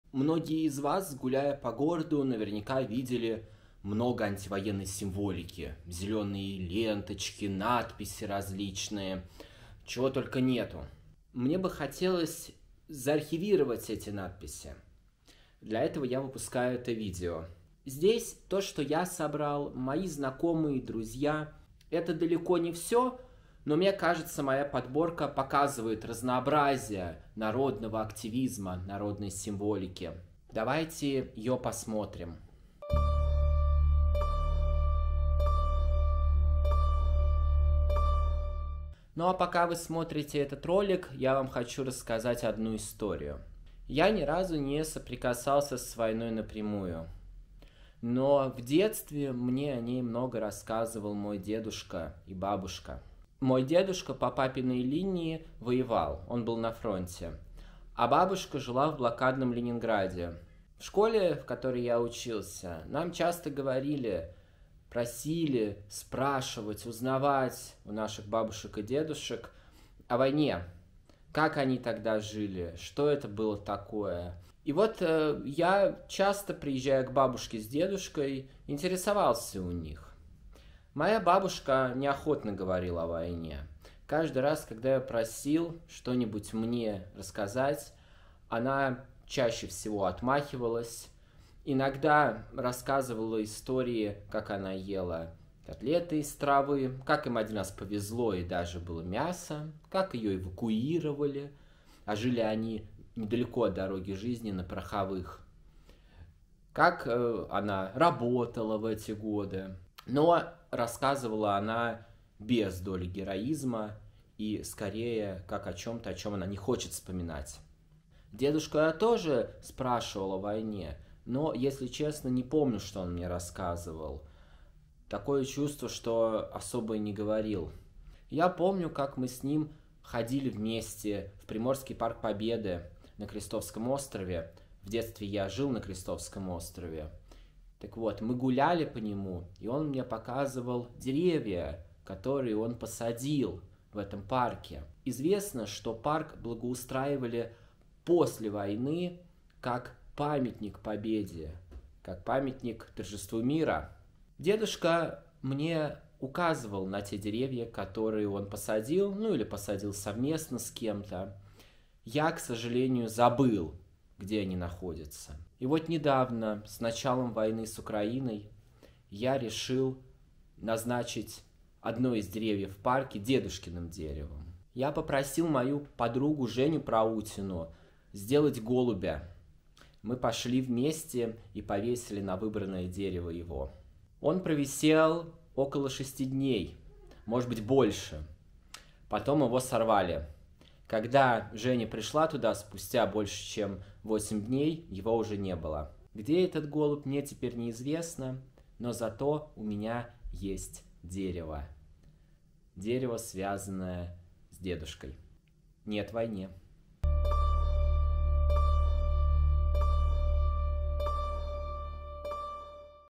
Антивоенный стрит-арт Петербурга. Нет войне.
Объединил в одном видео большое количество народного антивоенного стрит-арта. Эти фотографии сделаны моими друзьями и знакомыми в разных частях Петербурга.